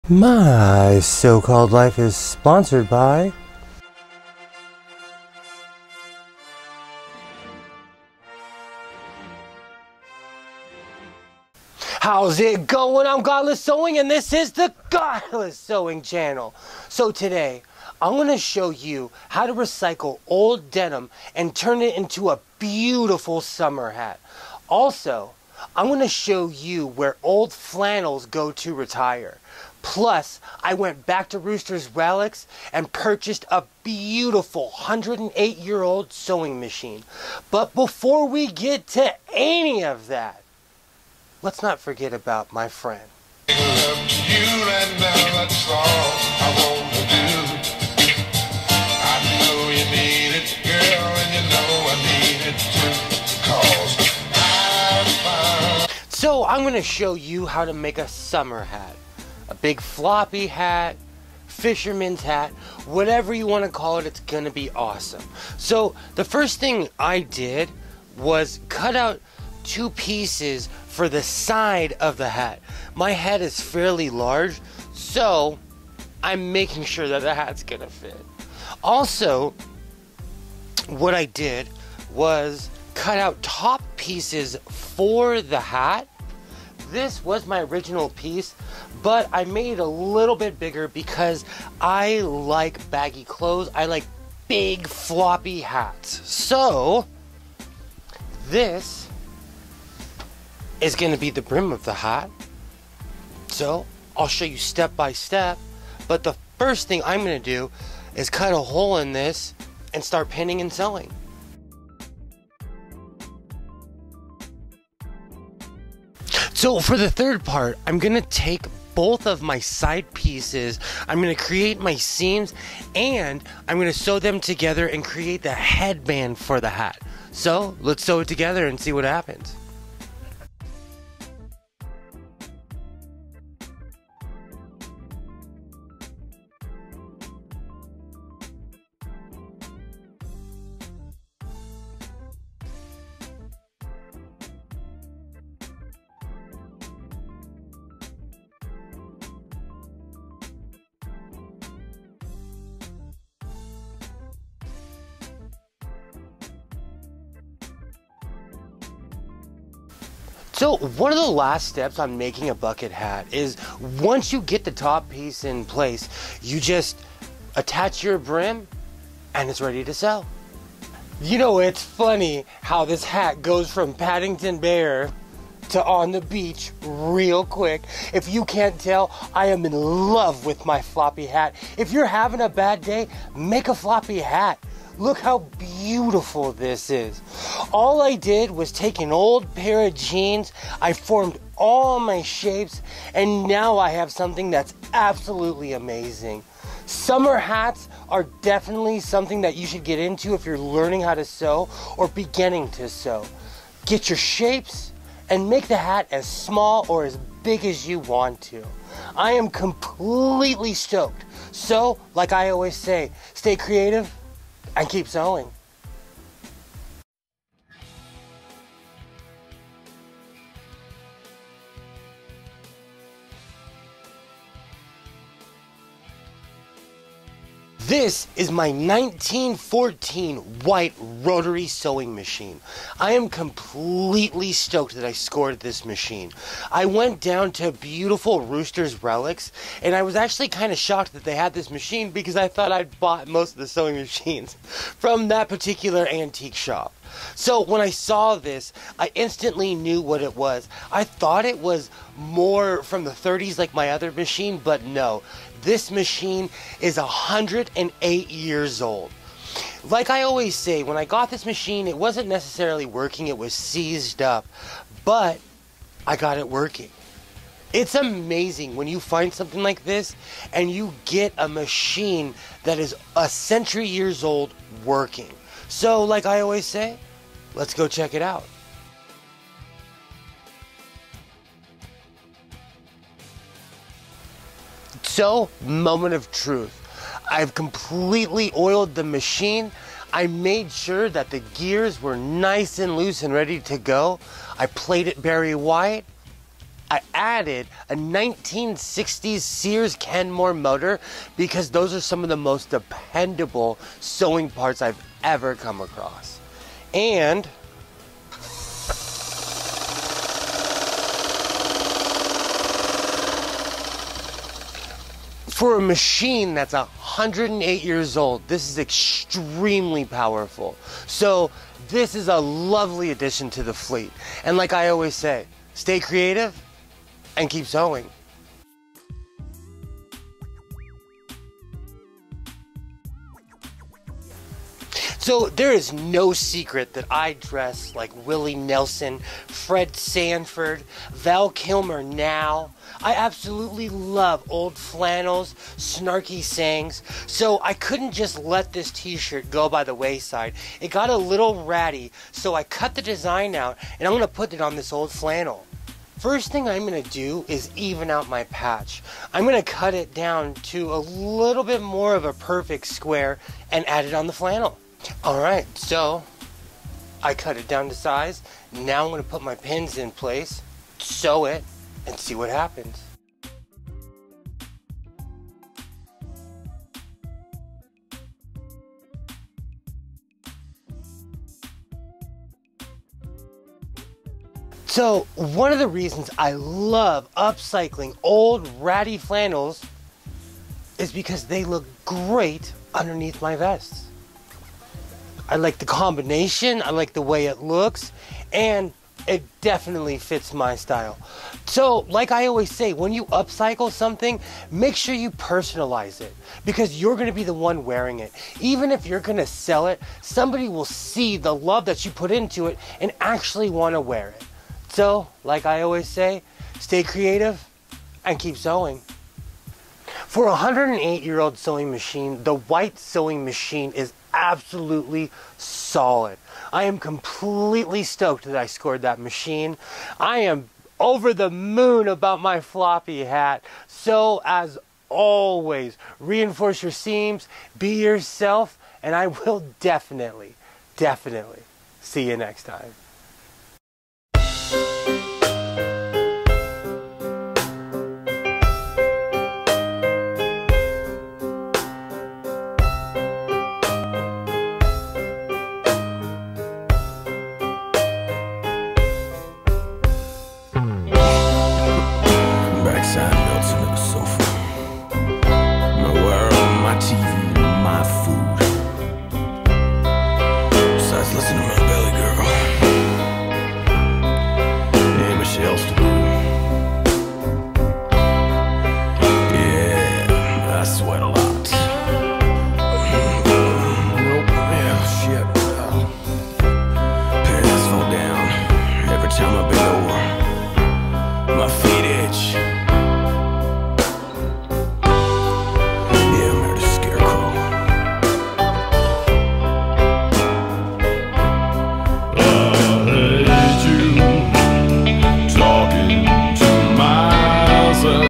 My Sew Called Life - Restoring A WHITE Sewing Machine From 1914 & The Bucket Hat Of Circumstance
In this installment of My Sew Called Life I had the fortune to restore a White Rotary Sewing Machine and explore the world of bucket hats and the mayhem that follows. As always, reinforce your seams and always be yourself for those who cannot.